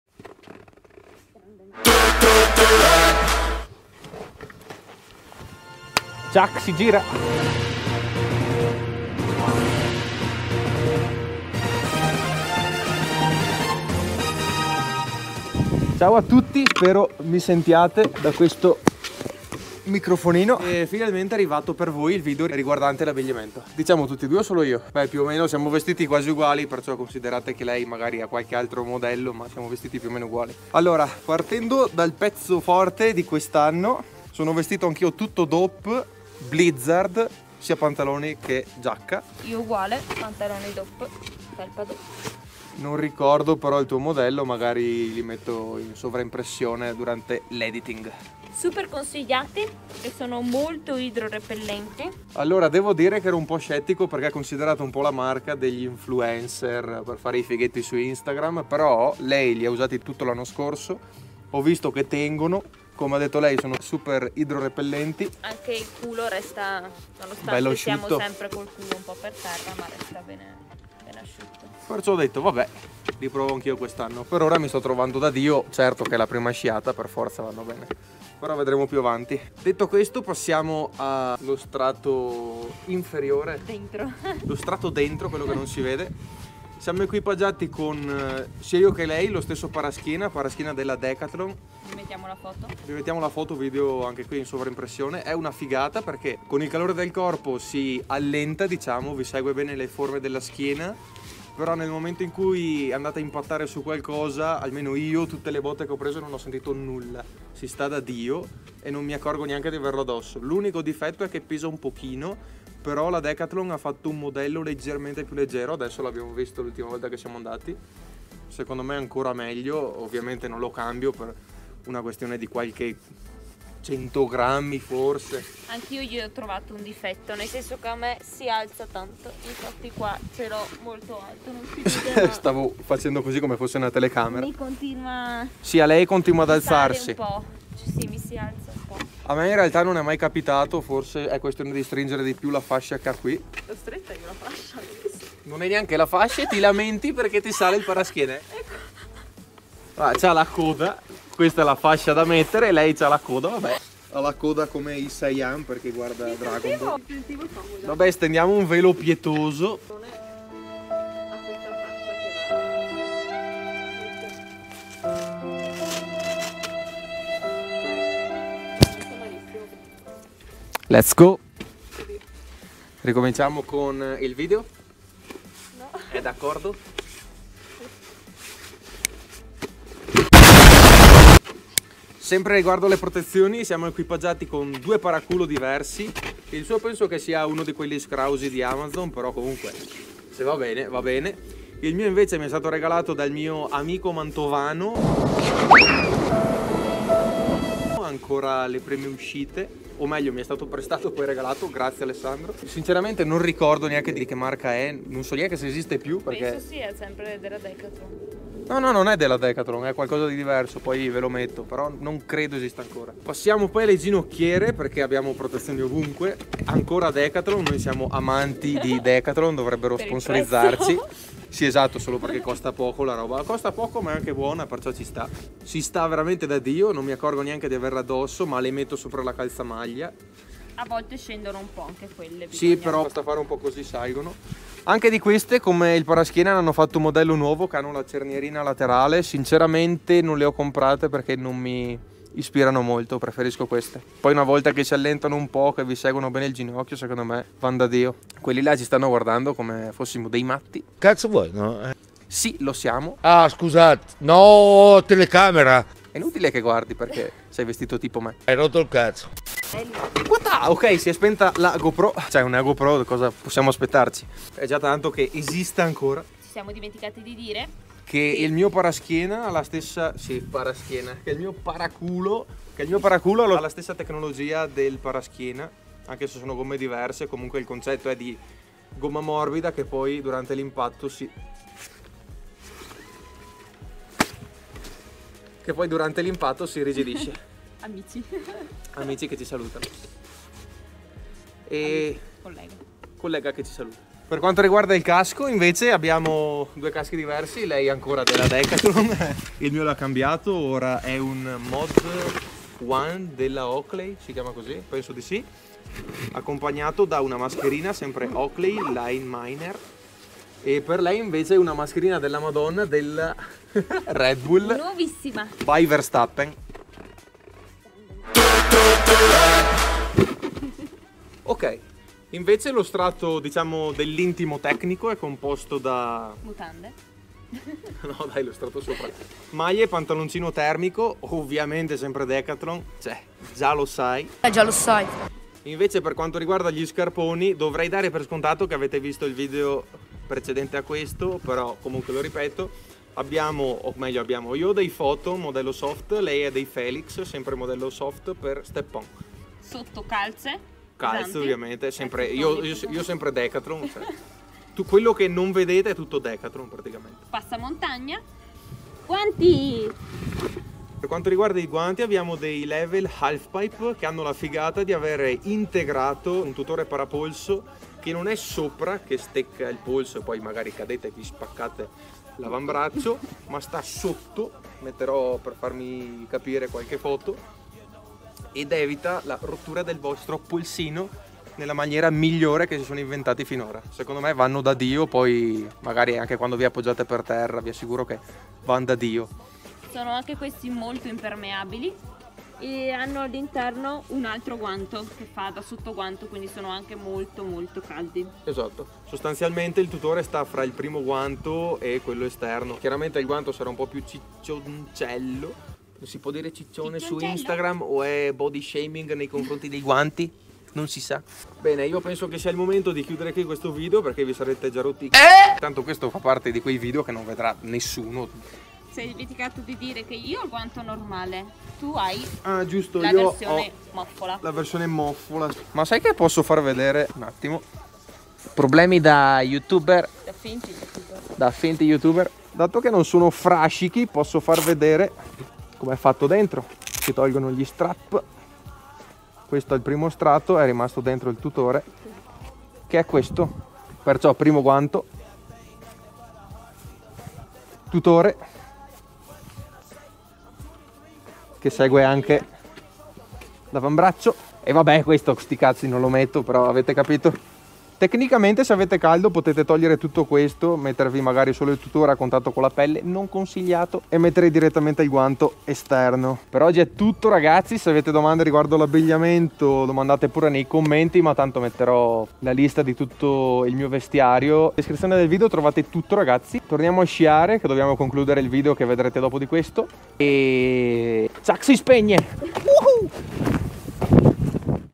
Il mio ABBIGLIAMENTO da SNOWBOARD | VLOG
MyPass Ski codice promo:  TOMI26
Il mio abbigliamento da snowboard, non sottovalutate le protezioni:
- Moffole Level Half Pipe
- SanDisk Extreme Pro 128 GB
- Dc Shoes Dryden
- Oakley Line Miner
- Insta360 X3
- Hensych Supporto per casco in lega di alluminio